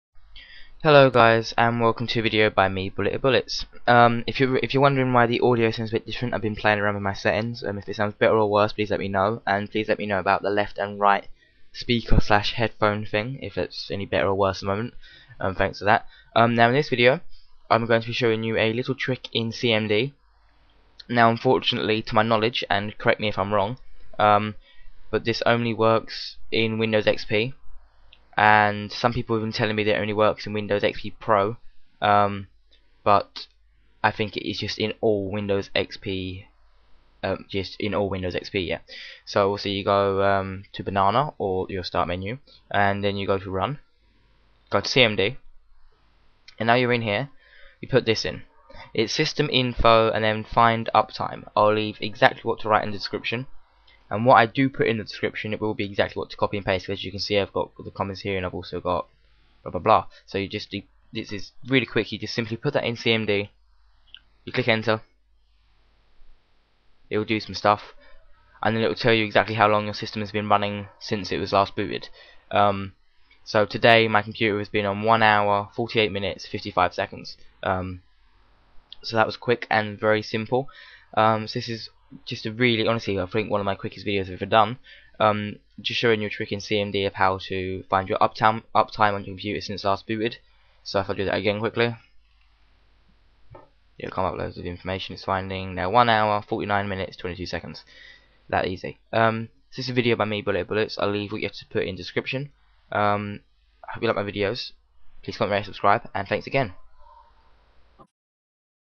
CMD - How To Find Your Systems Uptime
This video I show you how to find you Uptime on a XP system.

What you need to put in CMD: systeminfo | find "Up Time"